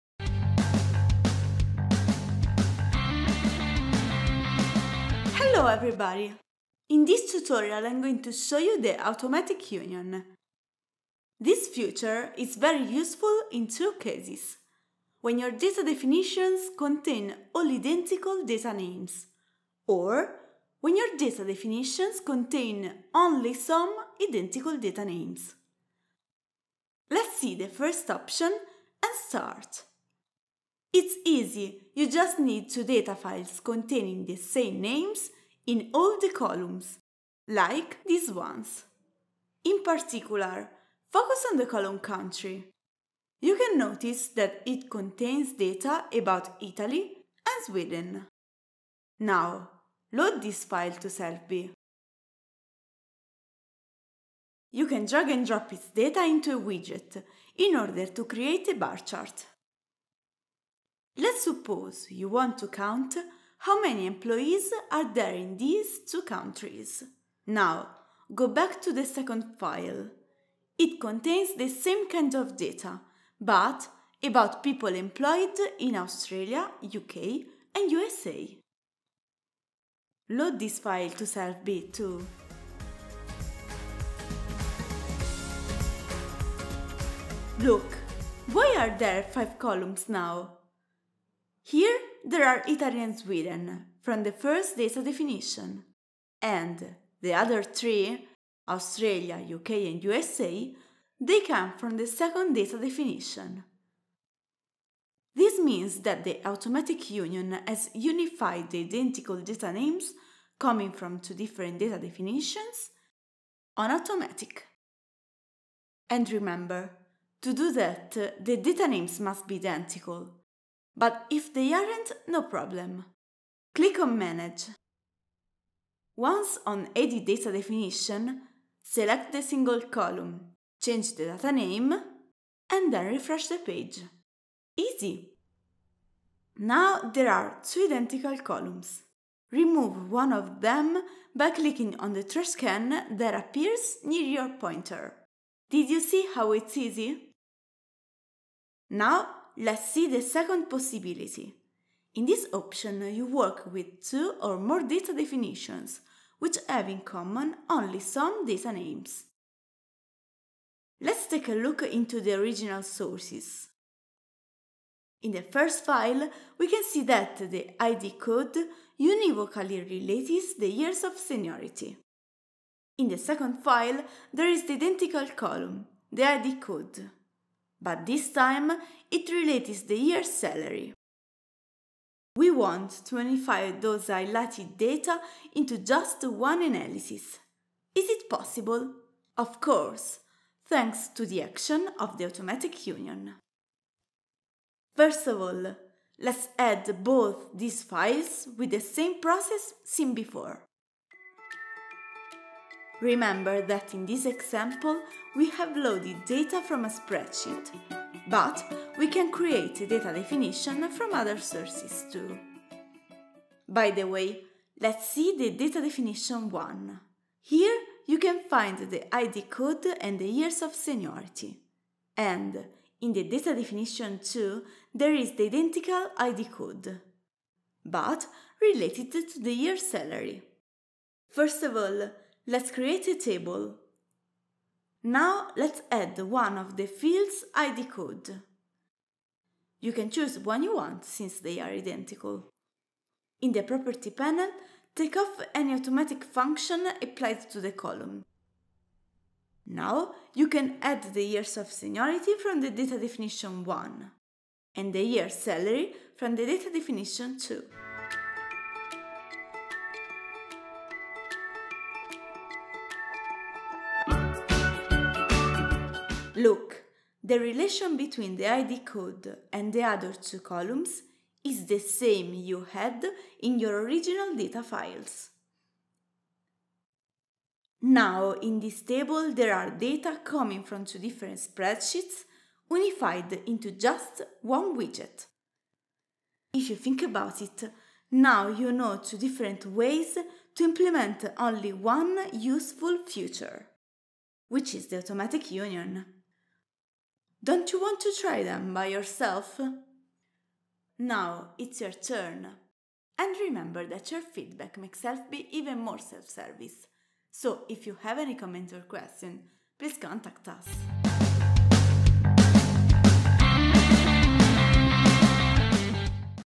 The Automatic Union ► SelfBi Tutorial #9
The Automatic Union is a smart and useful feature of SelfBi that allows you to analyse different data and data files.